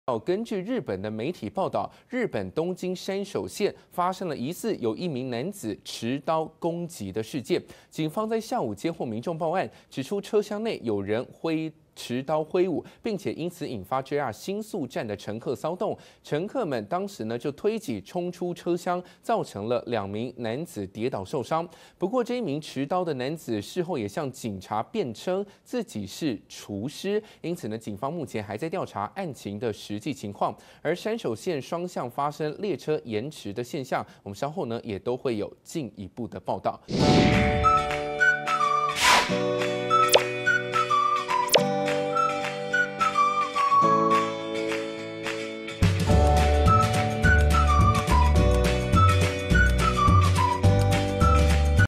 【每日必看】日本JR山手線有人持刀! 乘客竄逃 雙向延遲 20230625
根據日本媒體報導，日本東京山手線，發生疑似一名男子持刀攻擊事件，警方在下午接獲民眾報案，指出車廂內有人持刀揮舞，並因此引發JR新宿站乘客騷動，乘客們當時推擠衝出車廂，造成2名男子跌倒受傷，不過這名持刀男子事後向警察辯稱，自己是廚師。因此警方目前還在調查，案情的實際情況。